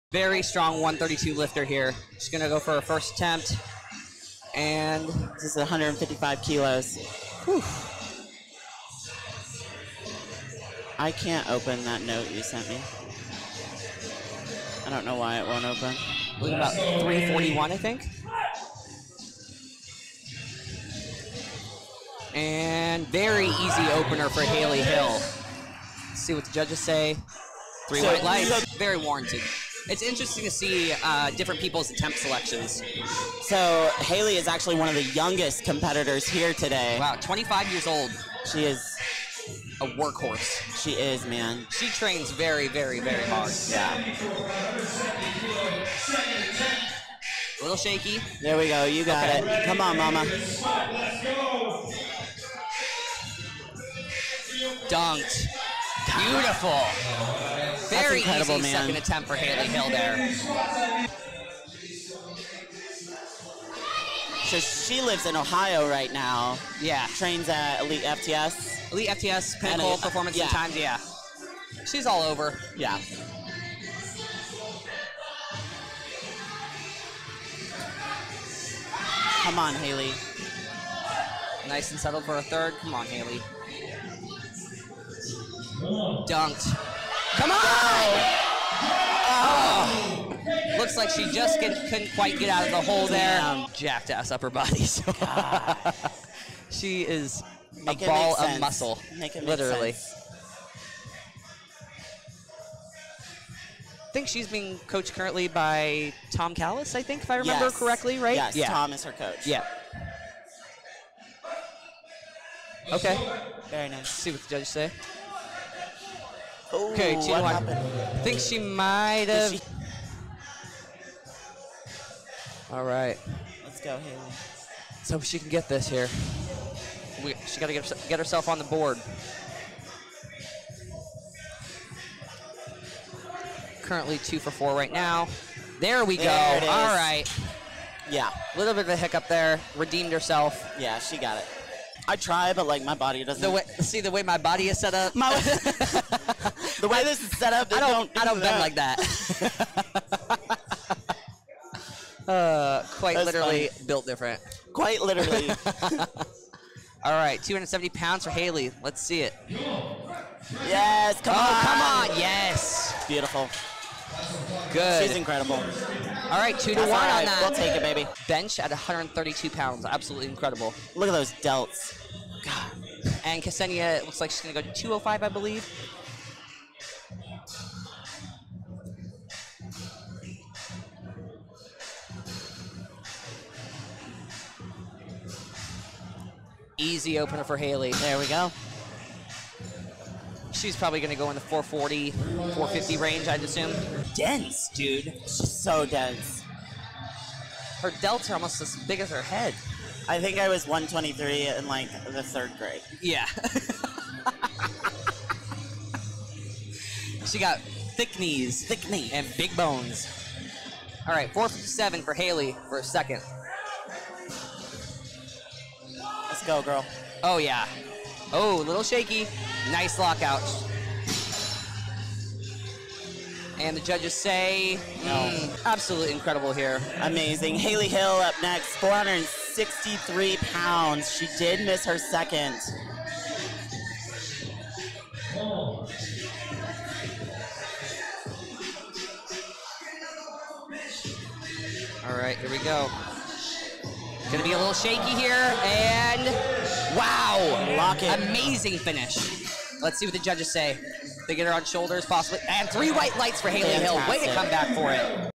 Haley Hill • 1st place 502.5kg Total • 60kg Class • USPA Pro Raw 2022
Haley Hill
1st place 502.5kg Total
60kg Class
USPA Pro Raw 2022